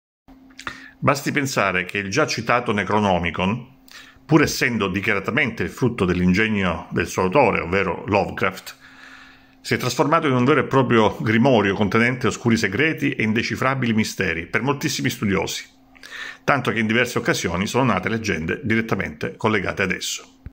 L’universo al confine della paura: la vita e le opere di H.P. Lovecraft di Stefano Donno
“Non vi è un solo narratore di horror, fantascienza, weird, soprannaturale, grottesco – sia contemporaneo che successivo a H.P. Lovecraft – che non si sia ad egli ispirato: nomi tra i quali si annoverano Stephen King, George R.R. Martin, Robert McCammon e Joyce Carol Oates devono, alla loro letteratura, la forte ispirazione ricevuta da Howard Philipp Lovecraft, maestro comune che oggi è e resta immortale, tra i più amati e letti di tutti i tempi. (dalla prefazione di Antonia Depalma)

(…) Basti pensare che il già citato Necronomicon, pur essendo dichia- ratamente il frutto dell'ingegno del suo autore, si è trasformato in un vero e proprio grimorio contenente oscuri segreti e indecifrabili misteri per moltissimi studiosi, tanto che in diverse occasioni sono nate leggende direttamente collegate ad esso. (dalla post fazione di Mario Contino)